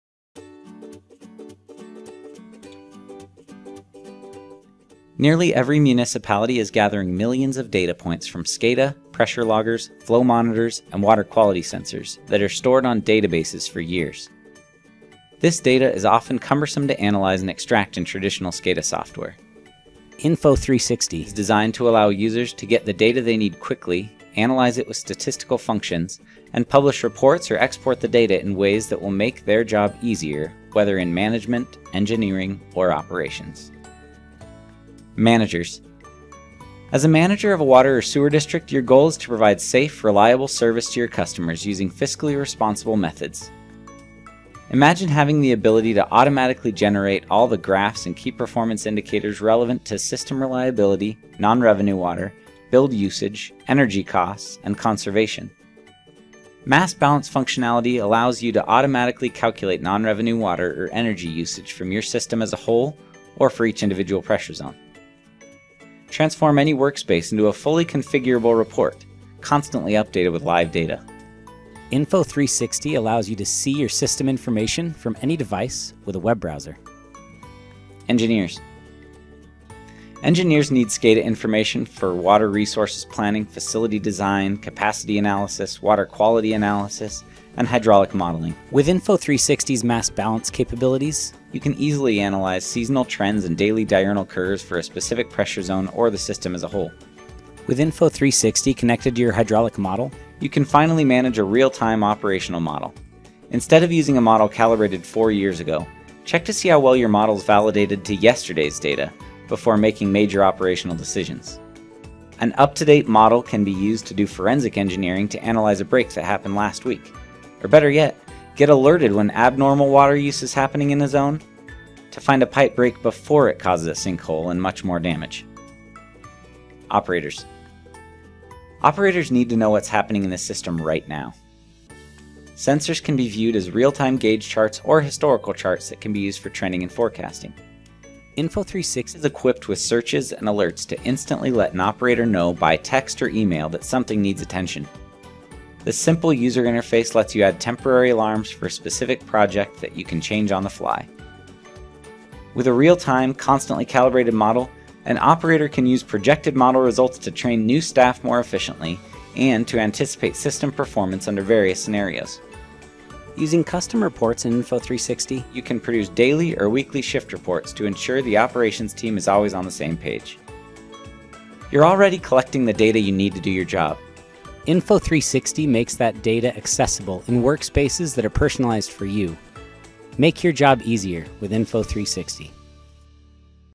Info360 Overview
An overview of the latest in operational analytics from Innovyze. Introducing Info360.